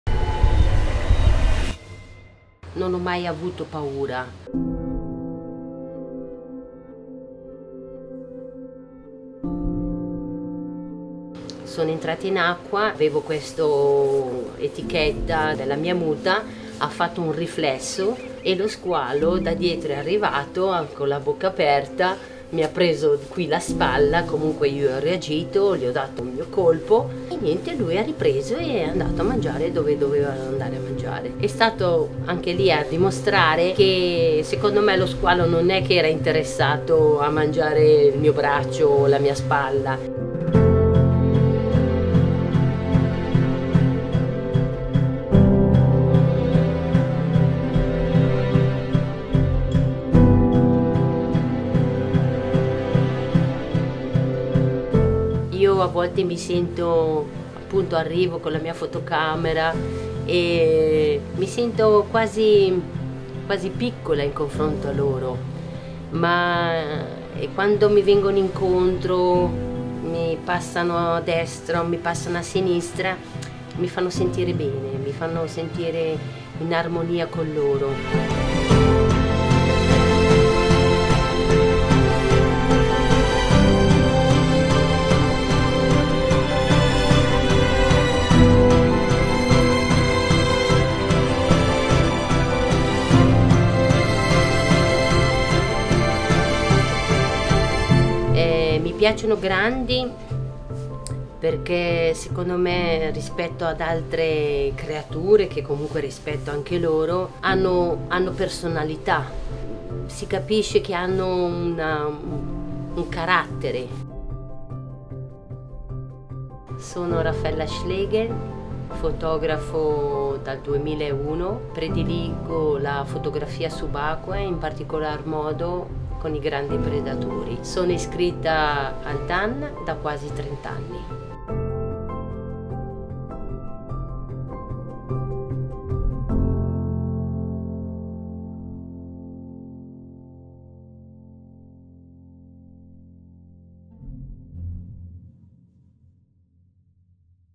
Underwater Photographer Raffaella Schlegel
Raffaella’s journeys take her around the world to some of the most extraordinary places. As a photographer, she enjoys the exclusive privilege of being in close proximity to wild sea creatures. The ocean, the light and the energy found underwater has a unique story to tell, which Raffaella manages to capture in incredible ways. When freediving, the deeper she goes, the more she discovers.